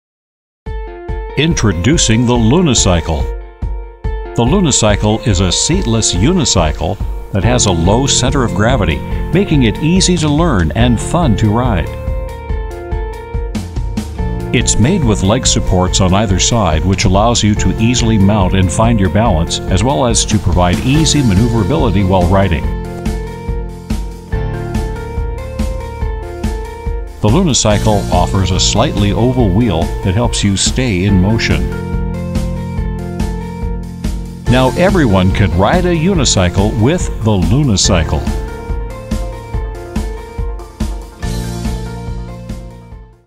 Introducing the Lunicycle
A seat-less unicycle that is easy to learn and fun to ride.